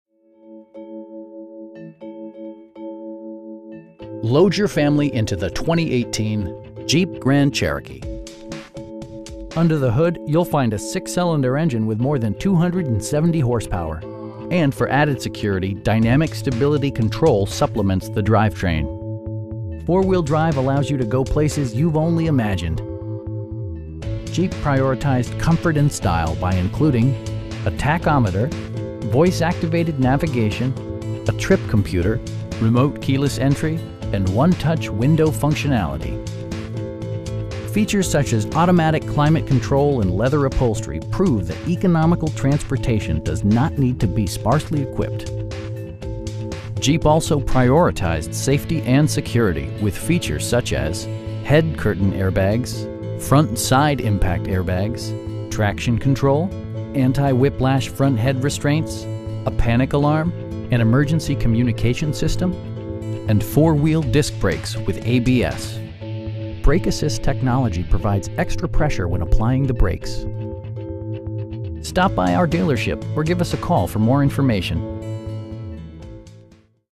2018 Jeep Grand Cherokee Laredo 4x4 in San Diego, CA 92108
777 Camino Del Rio South

Load your family into the 2018 Jeep Grand Cherokee. Under the hood you'll find a 6 cylinder engine with more than 270 horsepower, and for added security, dynamic Stability Control supplements the drivetrain. Four wheel drive allows you to go places you've only imagined. Jeep prioritized comfort and style by including: a tachometer, voice activated navigation, a trip computer, power windows, remote keyless entry, an overhead console, and 1-touch window functionality. Features such as automatic climate control and leather upholstery prove that economical transportation does not need to be sparsely equipped. Jeep also prioritized safety and security with features such as: head curtain airbags, front side impact airbags, traction control, anti-whiplash front head restraints, a panic alarm, an emergency communication system, and 4 wheel disc brakes with ABS. Brake assist technology provides extra pressure when applying the brakes. We know that you have high expectations, and we enjoy the challenge of meeting and exceeding them! Stop by our dealership or give us a call for more information.